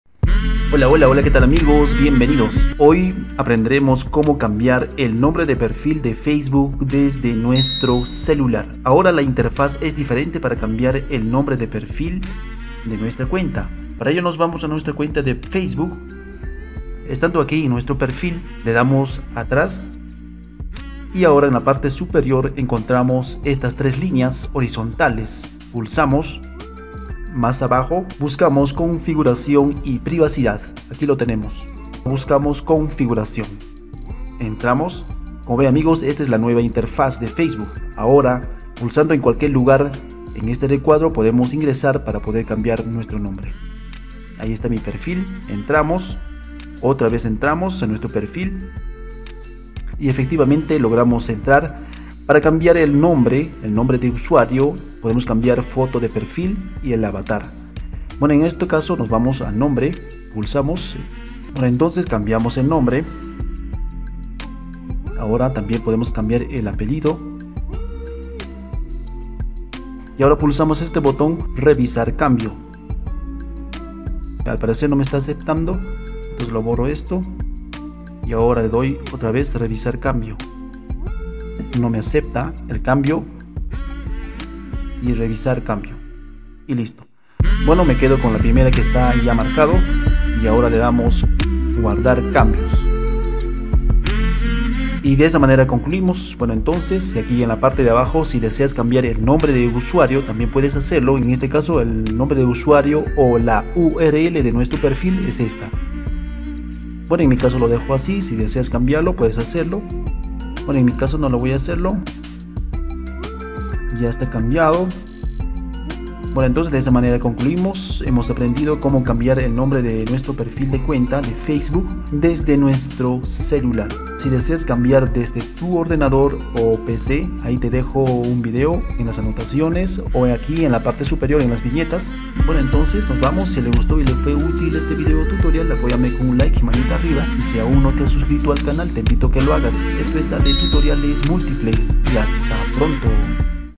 Como cambiar el NOMBRE de perfil de FACEBOOK desde Celular
✅  Tutorial Cambiar o editar el nombre de perfil de Facebook desde celular. Ahora el procedimiento es diferente para poder cambiar el nombre de perfil de Facebook 2024
Espero que hayan alcanzado sus propósitos al ver este vídeo de ALE TUTORIALES MULTIPLES, ¡ABRAZOS Y MUCHOS ÉXITOS!